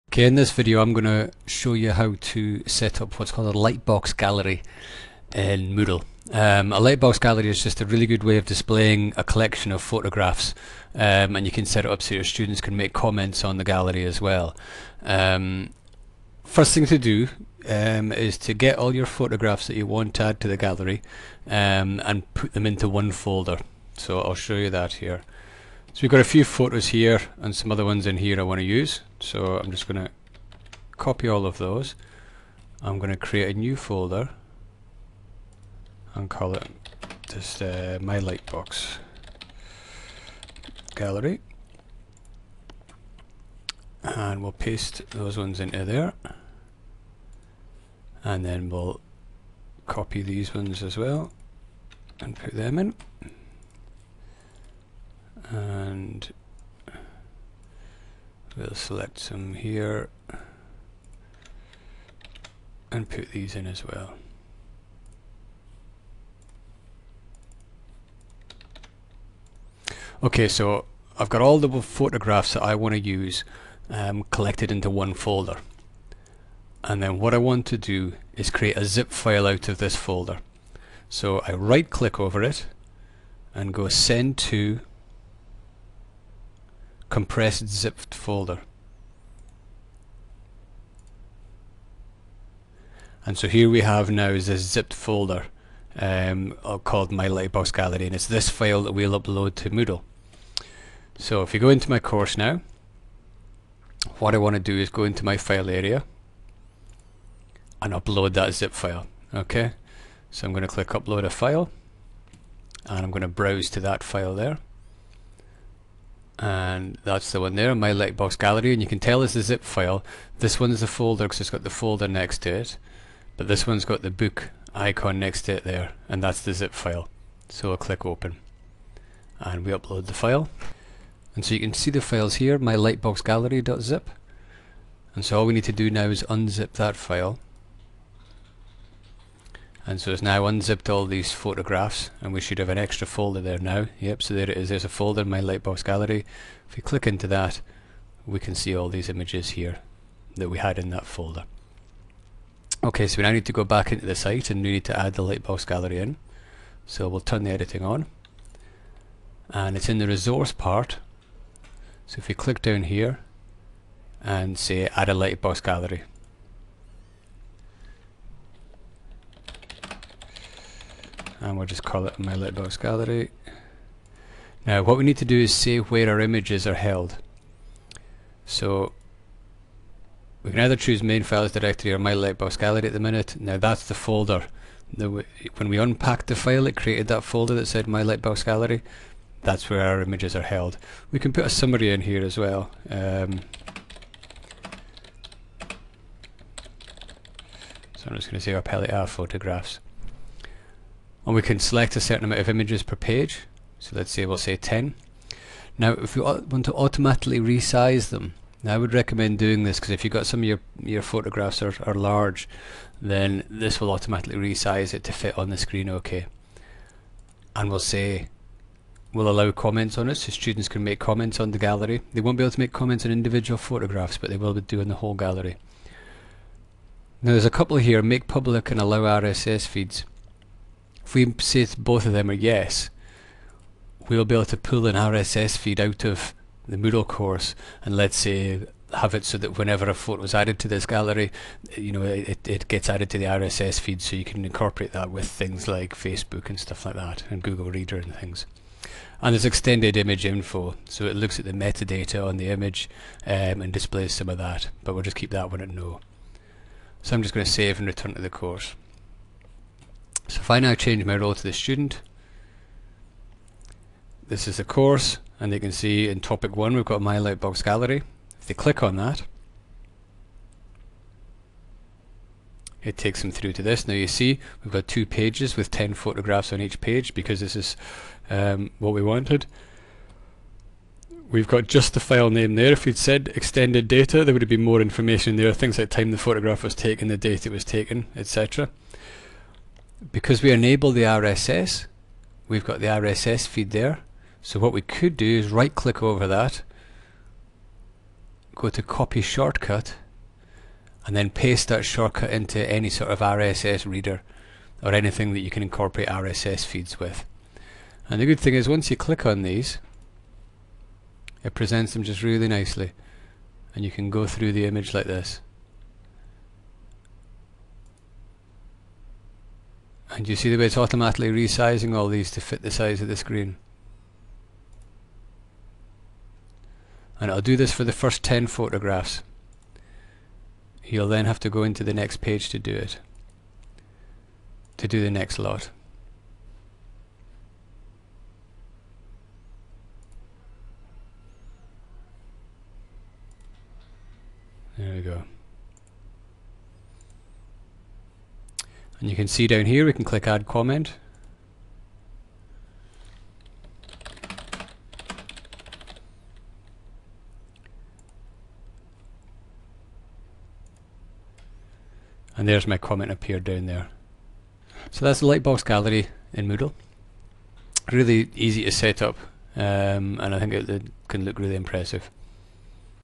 LightboxGallery.mp4
How to use lightbox gallery in Moodle.